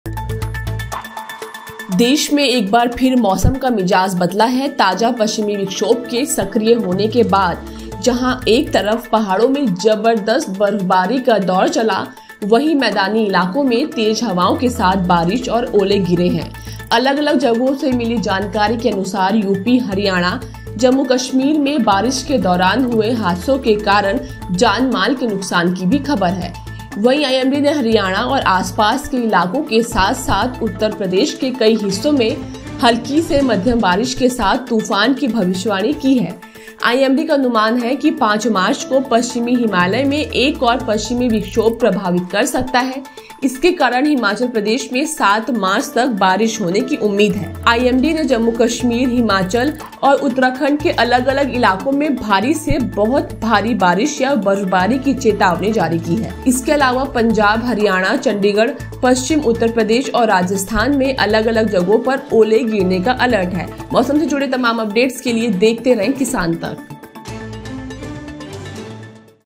breakingnews | India Weather Today: देश के इन राज्यों में अगले दो दिन Heavy Rain का Alert | IMD
देश में एक बार फिर मौसम का मिजाज बदला है. ताजा पश्चिमी विक्षोभ के संक्रिय होने के बाद जहां एक तरफ पहाड़ों में जबदरदस्त बर्फबारी का दौर चला है वहीं मैदानी इलाकों में तेज हवाओं के साथ बारिश और ओले गिरे हैं. अलग-अलग जगहों से मिली जानकारी के अनुसार यूपी, हरियाणा और जम्मू-कश्मीर में बारिश के दौरान हुए हादसों के कारण जानमाल के नुकसान की भी खबर है... वहीं आईएमडी ने हरियाणा और आसपास के इलाकों के साथ-साथ उत्तर प्रदेश के कई हिस्सों में हल्की से मध्यम बारिश के साथ तूफान की भविष्यवाणी की है.